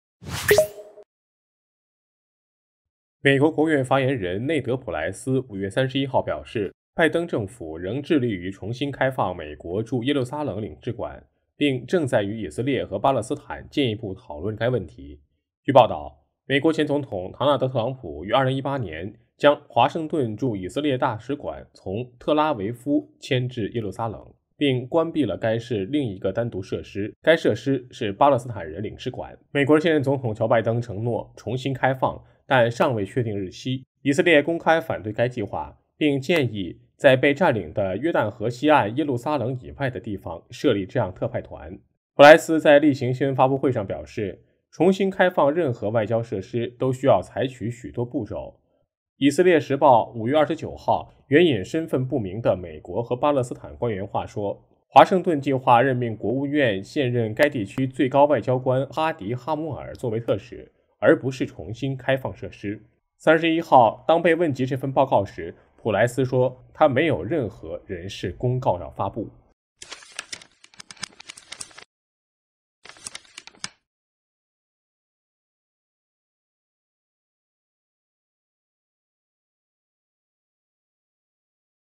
美国务院称致力于重开耶路撒冷领馆
国务院发言人内德·普莱斯周二（5 月 31 日）表示，拜登政府仍致力于重新开放美国驻耶路撒冷领事馆，并正在与以色列和巴勒斯坦进一步讨论这个问题。